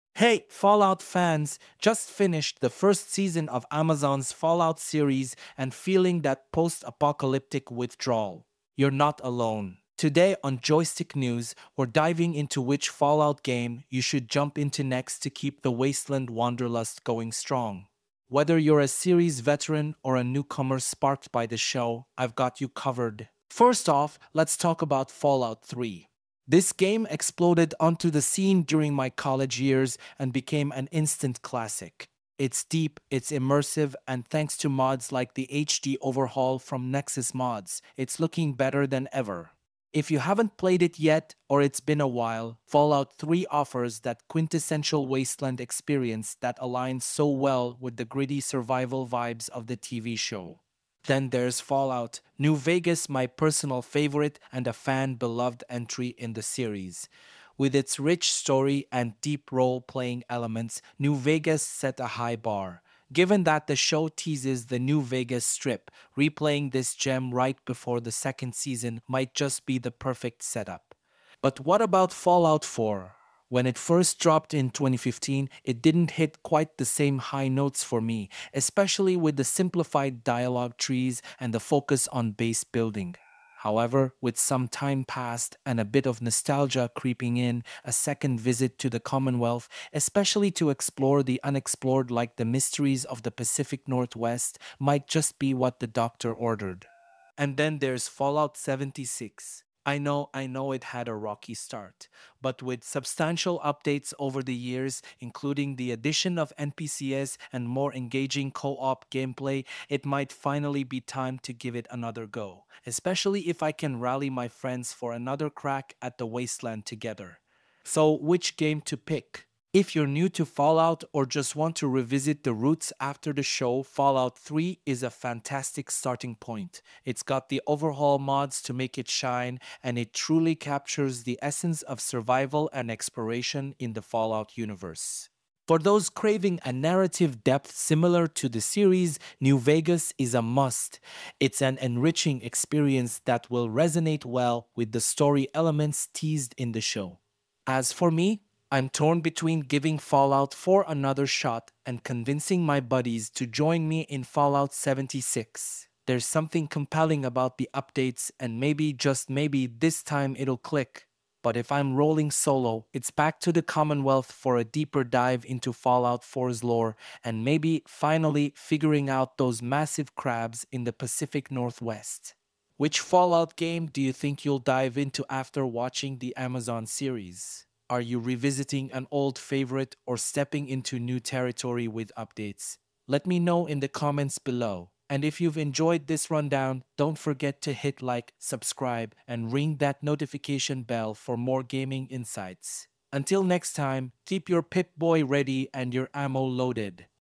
Best Fallout Game to Play After the Amazon Series? Find Out Now!
Can't get enough of the Fallout universe after watching the Amazon series? Dive into our latest video where we explore which Fallout game you should play next to keep the adventure going! We break down the pros and cons of Fallout 3, New Vegas, Fallout 4, and Fallout 76 based on gameplay, story alignment with the series, and recent updates. Perfect for newcomers and returning vault dwellers alike.